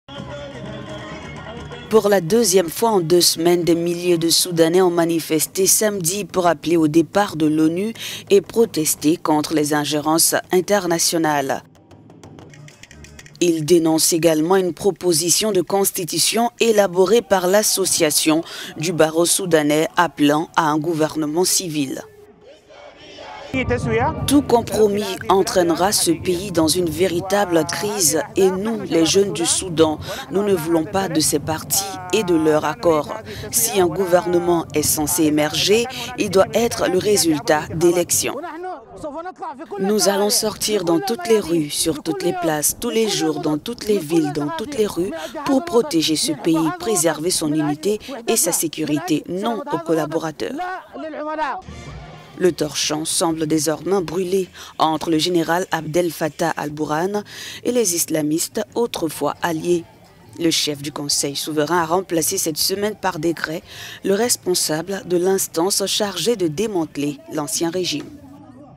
Des milliers de Soudanais manifestent contre l'ONU à Khartoum
"On manifeste contre l'ingérence étrangère dans notre pays, on n'acceptera pas la Constitution laïque du barreau soudanais", a lancé Ahmed Abderrahmane, un manifestant.